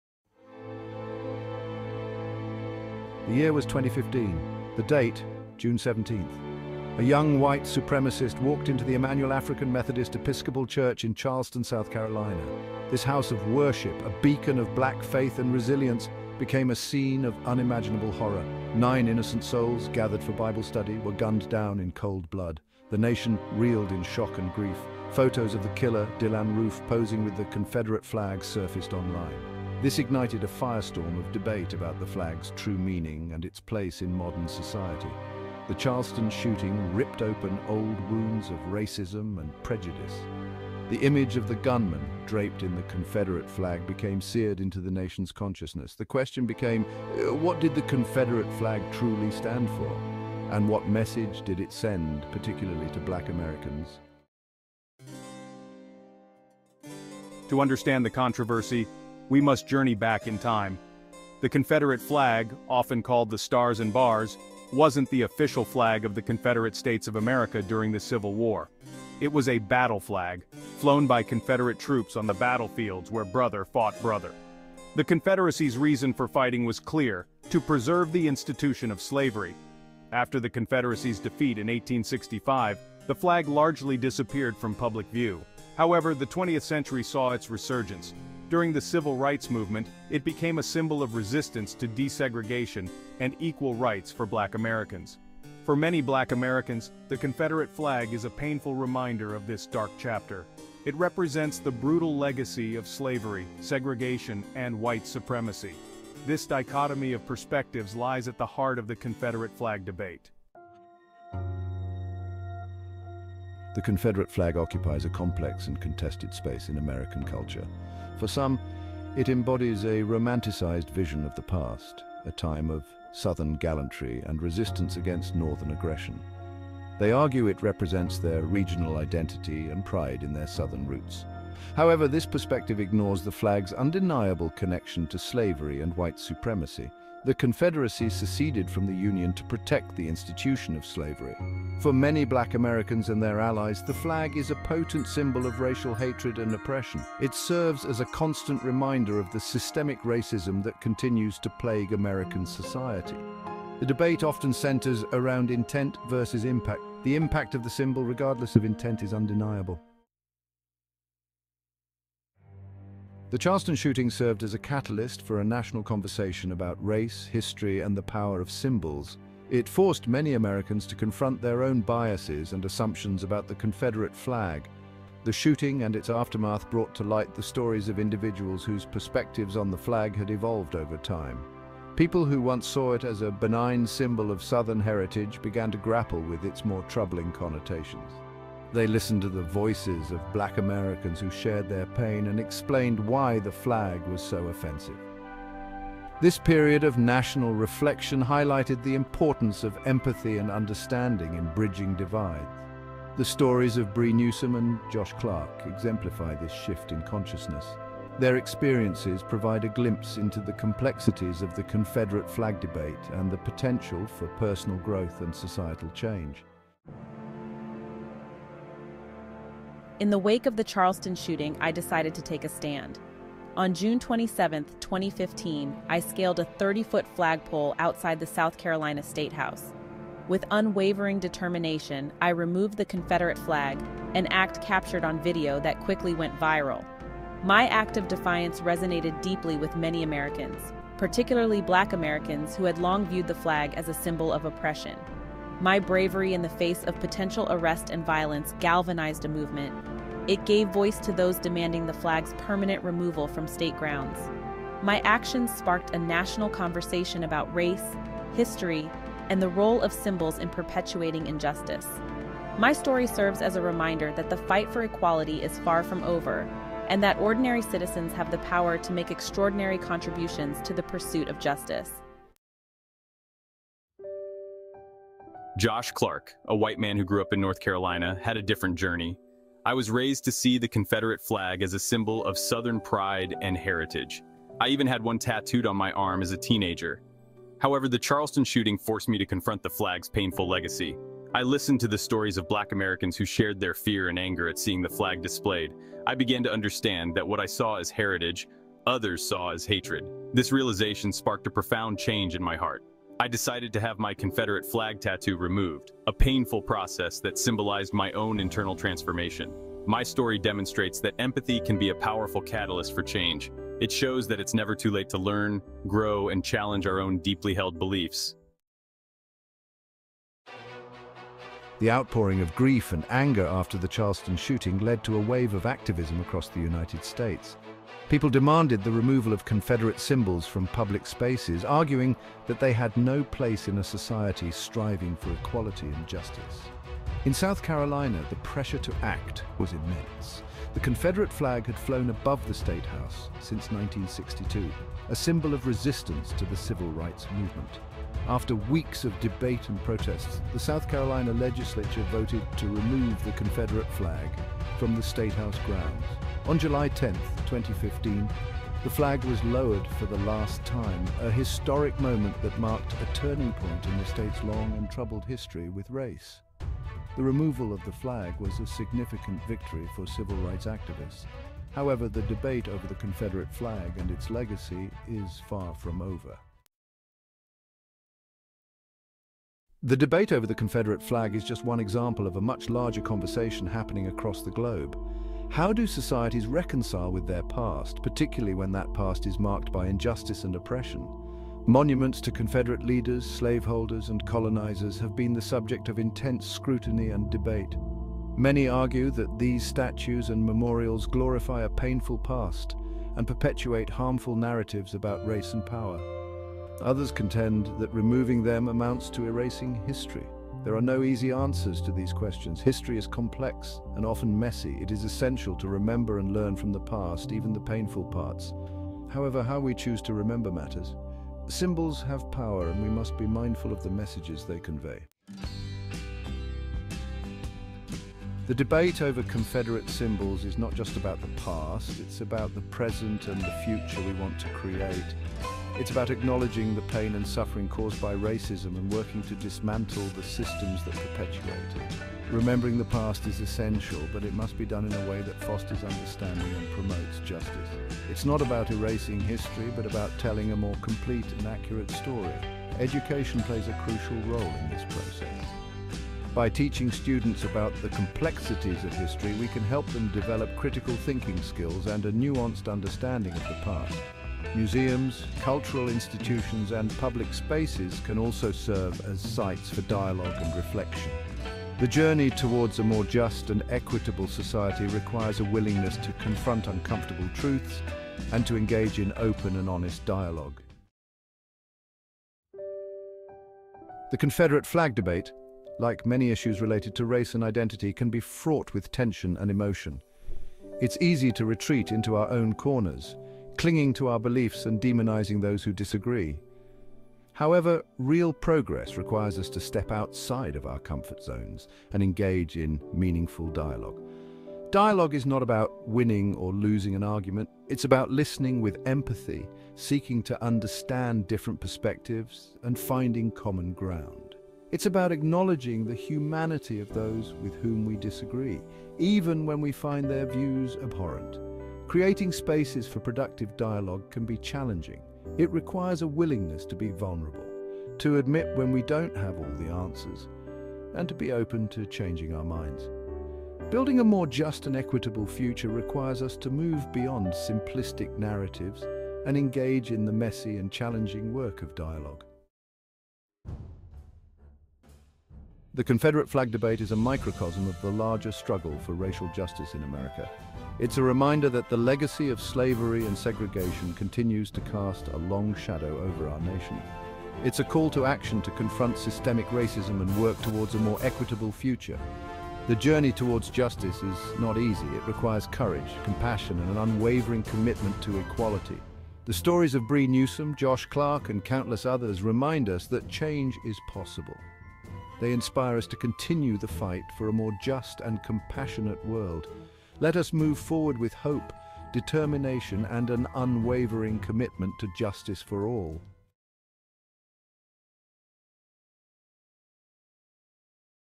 The Power of Symbols -The Confederate Flag
Delve into the world of symbols with Peachy King Productions as we explore the history and significance of the Confederate Flag. Join us for a discussion on symbolic flags, Southern heritage, and understanding black culture. Through comedy, satire, and commentary, this livestream sheds light on racism awareness and the need to stop racism. Tune in for a unique perspective on Confederate history and the impact of symbolic flags. Welcome to a journey of discovery with Peachy King Productions!